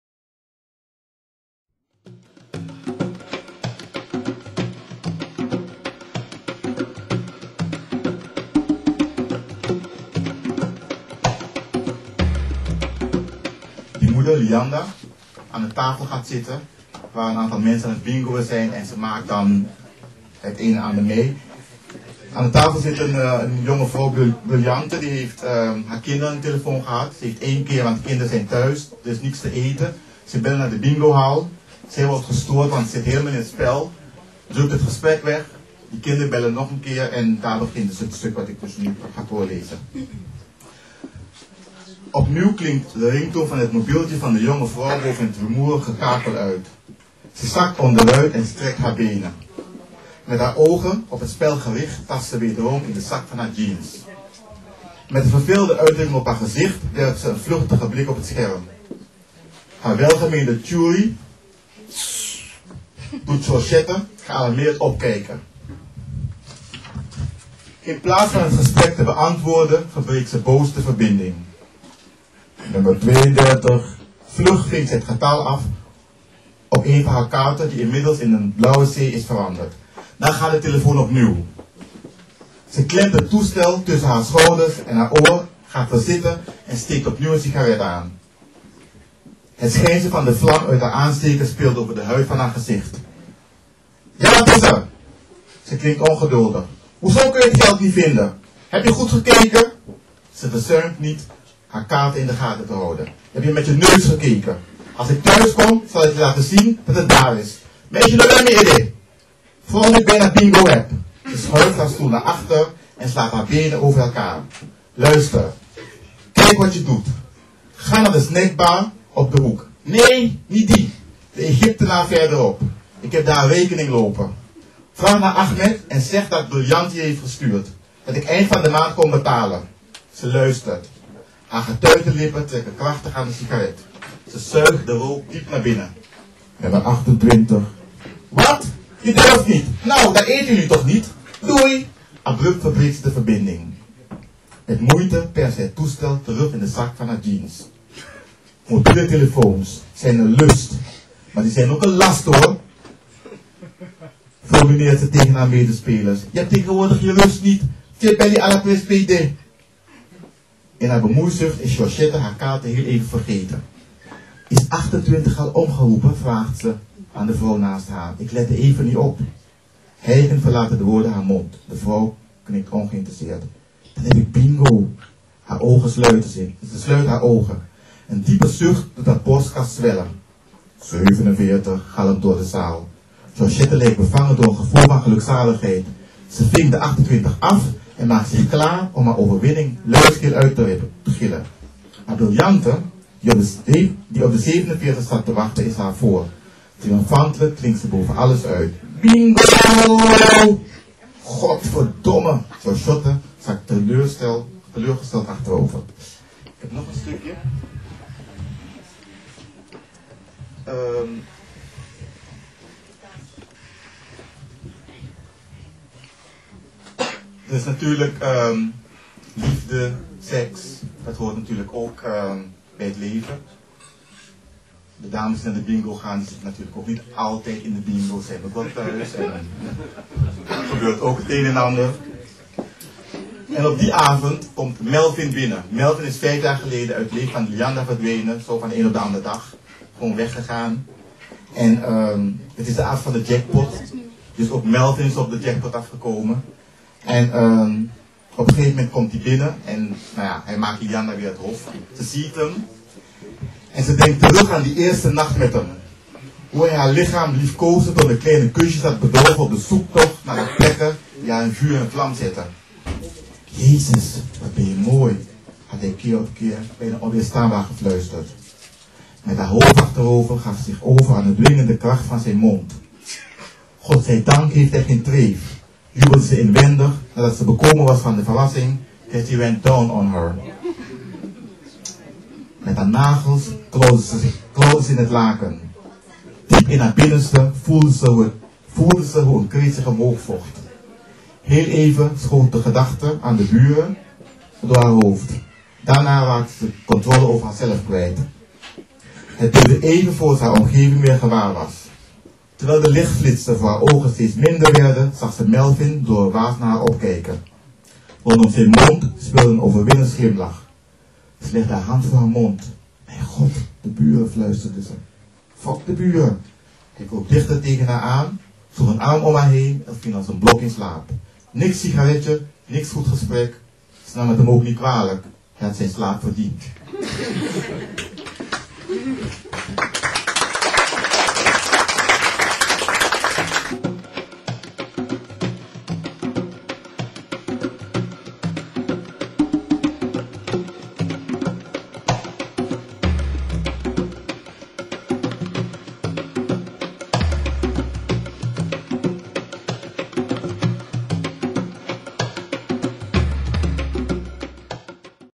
Clark Accord @ opening BLAAC Shop
writer and friend Clark Accord reciting from his latest novel Bingo.